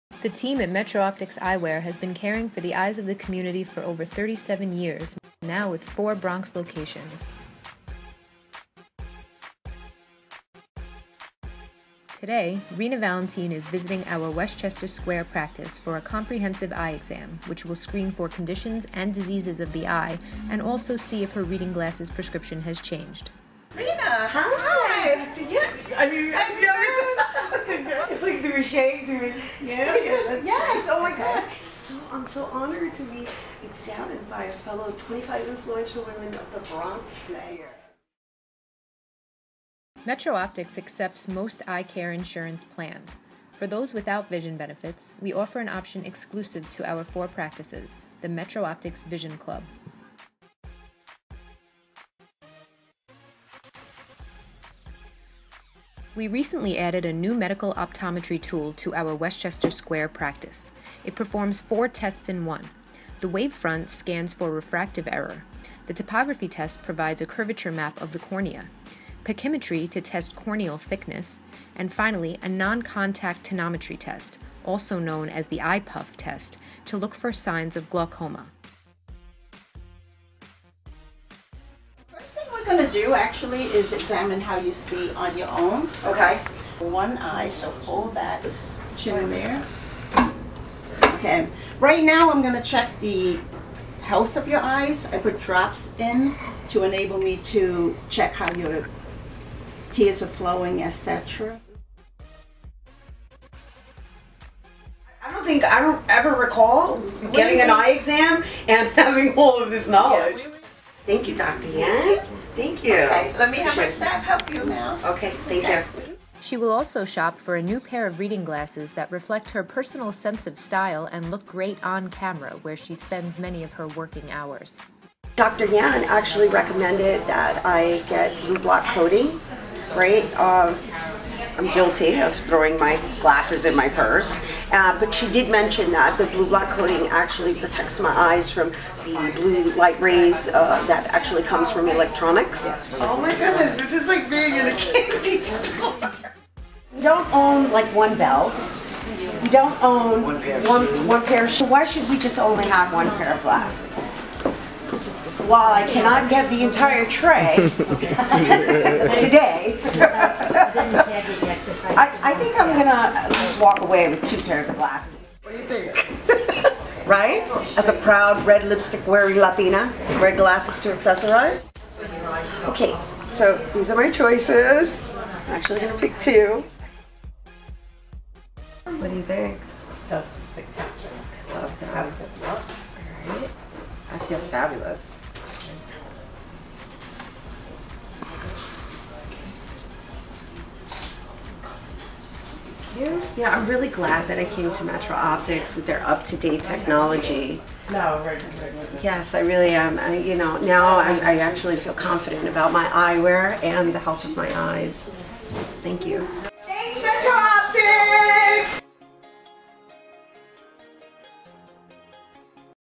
Rhina Gets Framed: Annual Eye Exam & New Glasses at Metro Optics
La Reina del Barrio, Rhina Valentin, visits our newly-updated Westchester Square location for her annual comprehensive eye exam, and picks out fabulous new designer glasses with custom lenses